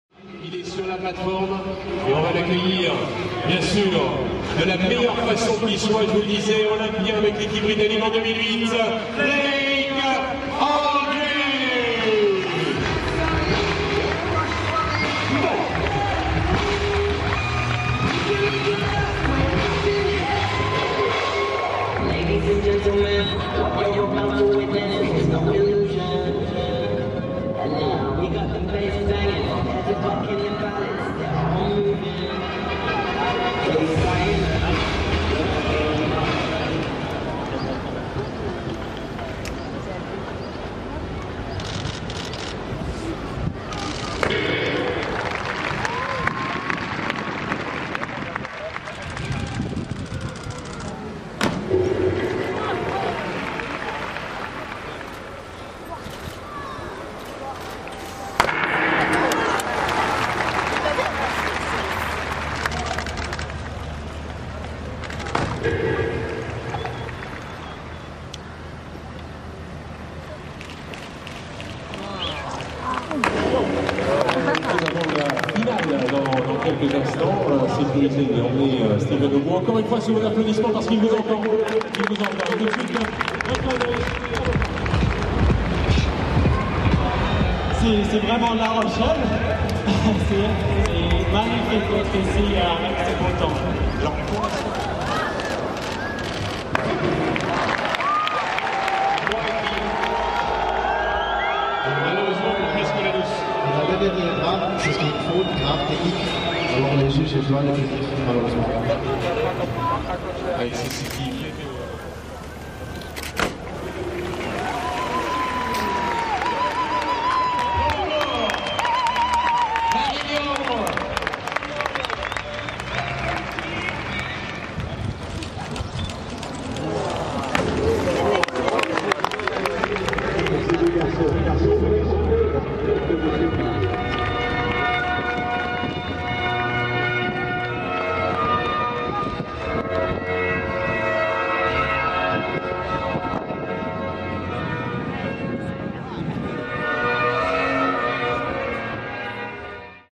Gary Hunt victorious of World Series's high flight contest
The English, Gary Hunt took the first place of Red Bull Diving World Series's high flight contest at La Rochelle, this week end on the 16 and 17 may, 2015, in front of his compatriot Blake Aldridge was very spectacular.